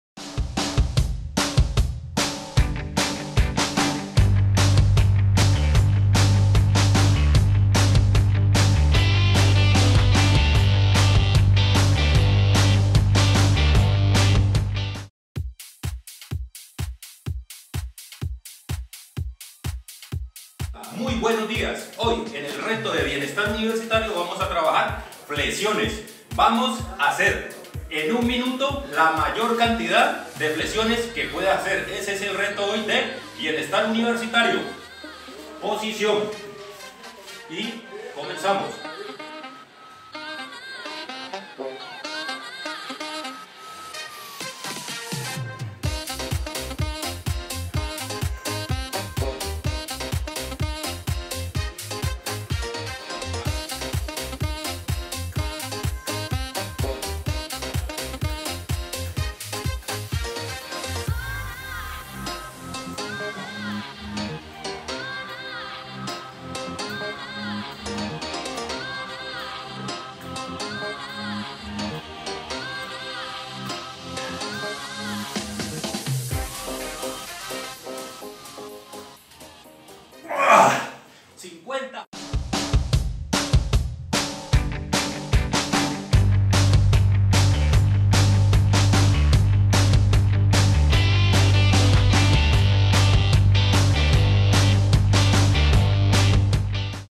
Reto flexiones de pecho
Desde la oficina de Bienestar Universitario y la coordinación de deporte invitan al reto flexiones de pecho.